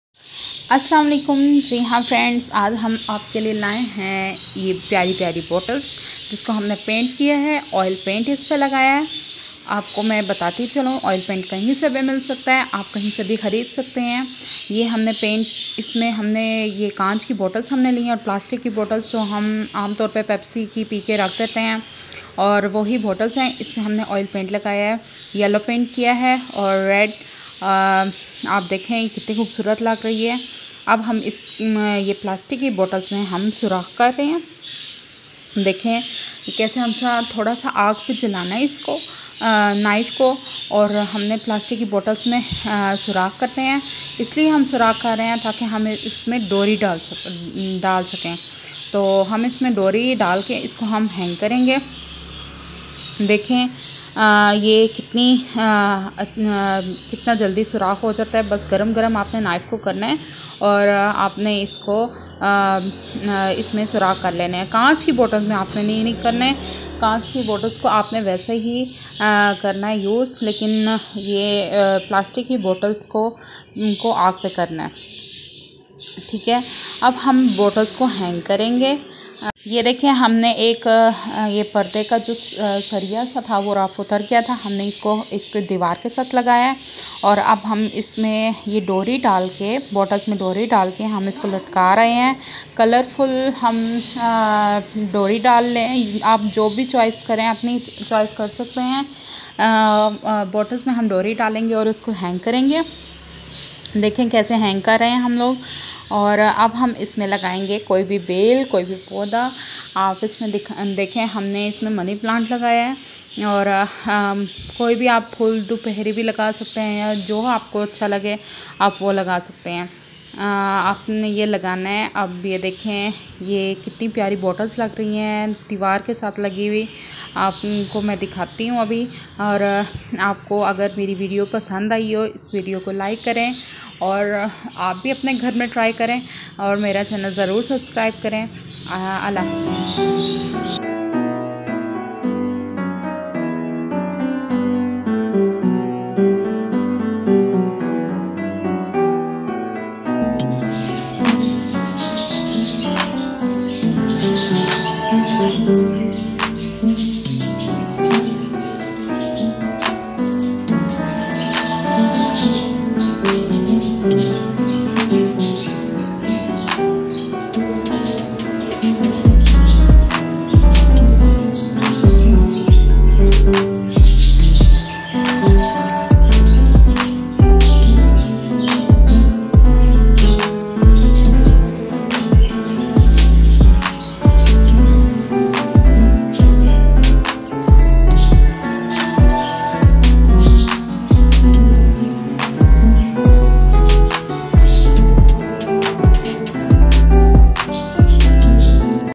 Oil Paint Bottles_Art world and talent
Ye bottles hen is per hum ne oil paint lgaya he phir is me surakh kr k dori ltkai he or phir is me pode lga k in ko hang kr dia he ye beautiful bottles ap b try kren